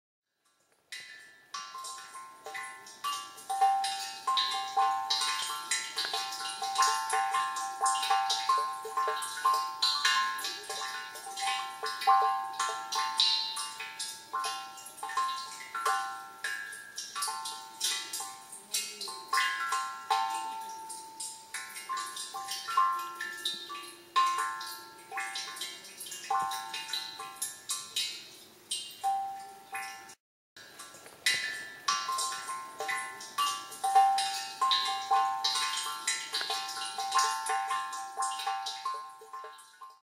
水琴窟リニューアル
口和ＩＣにあるもーもー物産館の「水琴窟」に人感センサーが設置されました、美しい癒しの音をお聞きください。